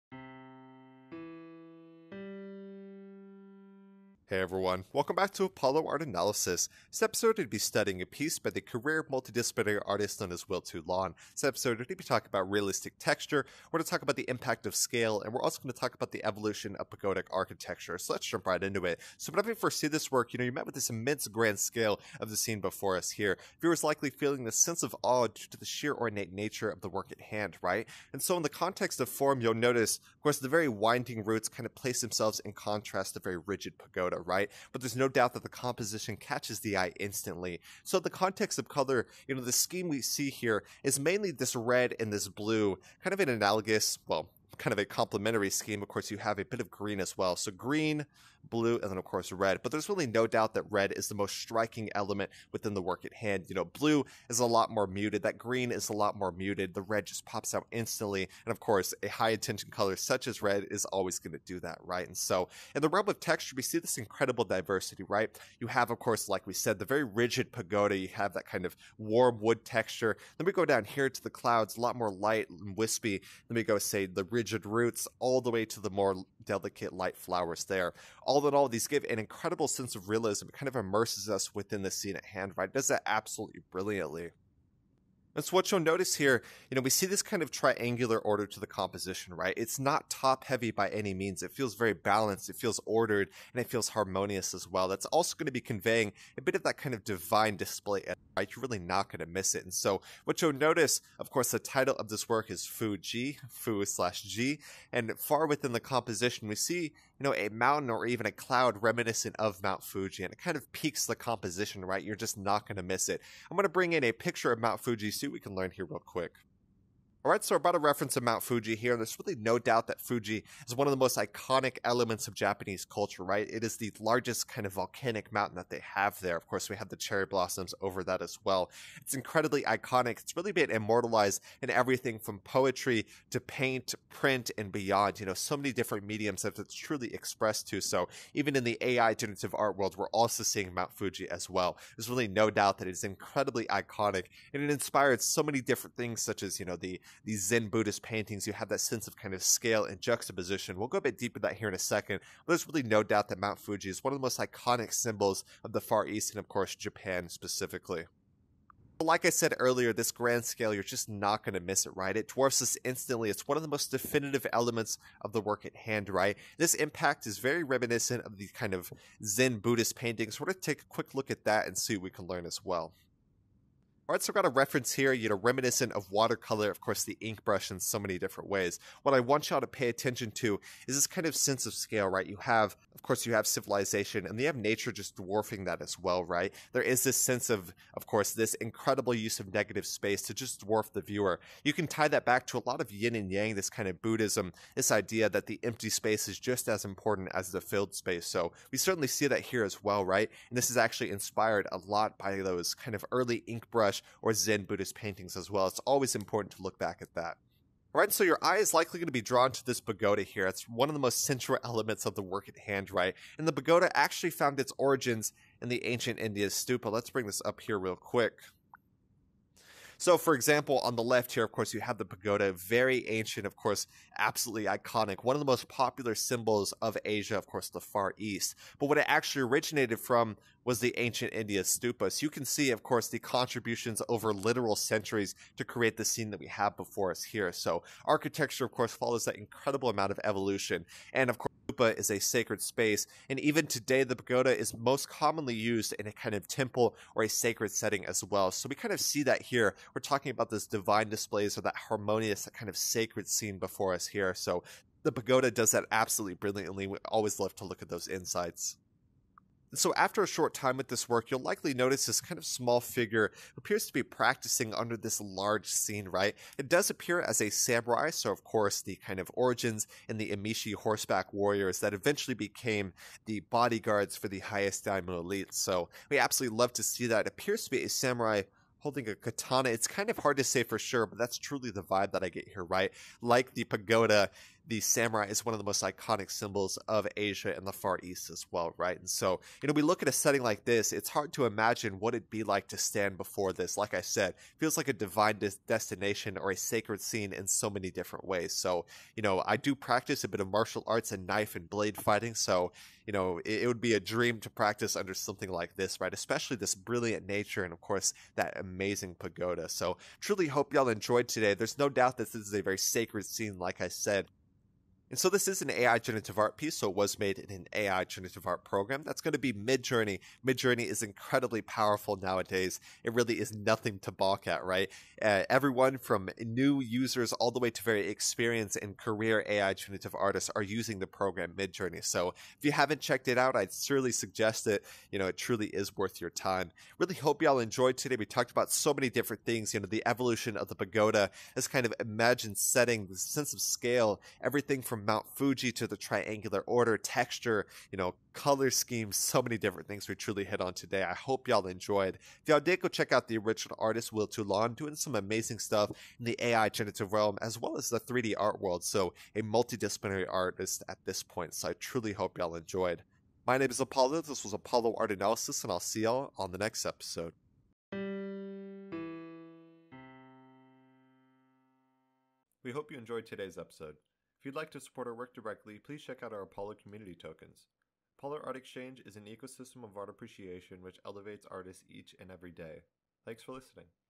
"Fu / Ji" - Will Toulan | Apollo Art Analysis
Created by Will Toulan, this divine AI Generative painting presents a lone samurai practicing under a large pagoda as inspirations of Zen Buddhist tradition make their masterful mark.

Set amidst the sacred scenes of the Far East is a powerful piece lending lessons on realistic texture, the impact of scale and the evolution of pagodic architecture.

At first sight we’re struck by the immense scale of the scene before us. The viewer is likely feeling a sense of ascendant awe due to the highly ornate nature of our piece.

Forms here are rather dynamic as both the pagoda and winding natural roots make their mark instantly within the composition.

In the context of color we note a complimentary scheme of red and blue feeling quite bold. Despite this it’s clear that the red is most vibrant as it draws our attention over blue.

When we take a look at texture the eye is drawn everywhere from the rougher roots to the lightest clouds and the rigid structure of our pagoda. All-in-all these immerse us through a masterful conveyance of realism.

Notice the overall silhouette of the combined elements in our canvas; Everything appears with a solid base in triangular order.

Far within the work before us appears to be a mountain or cloud reading reminiscent of Mount Fuji. This incredibly important Japanese symbol has been immortalized through countless mediums from theater to poetry, paint and print.

There’s no doubt that grand scale is one of the most definitive elements of this display. The impact of such scale is highly reminiscent of the zen buddhist style of painting which emphasizes negative space.

The large structure in the background is an Asian style of architecture known as a pagoda. Since its origins in the Buddhist stupa the pagoda continues to be used in the construction of sacred spaces.

After a short time with this work you’ll likely notice a small figure standing in the lower left region; This figure appears to be a samurai as one of the most iconic symbols of Japan subtly makes its mark.

Our setting is clearly one of the most beautiful sights we’ve seen. It’s hard to imagine what it’d be like to practice under a divine display such as this.

With all of this in mind we can’t help but point to the sacred as profound symbols of Japanese culture resurface in resounding order.

The work before us was created in the AI Generative art program known as Midjourney.

Will Toulan is a career multidisciplinary artist who made his premier debut in the Apollo Art Archive on the 28th of October in the year 2021.

Apollo Art Exchange is a community ecosystem and public art institution focused on the appreciation, cultivation and education of the arts in the digital ecosystem and beyond.